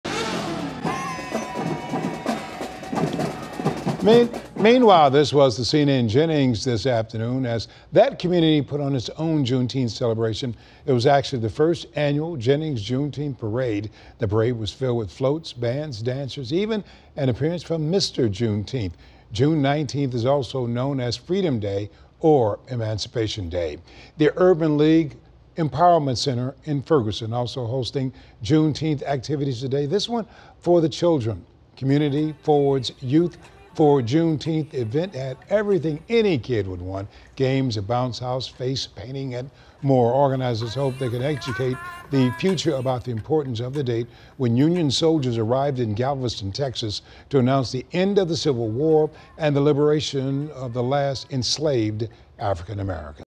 Jennings parade, Ferguson children's event mark Juneteenth
Community members all around the St. Louis region spent their Saturday afternoon celebrating Juneteenth ahead of the federal holiday.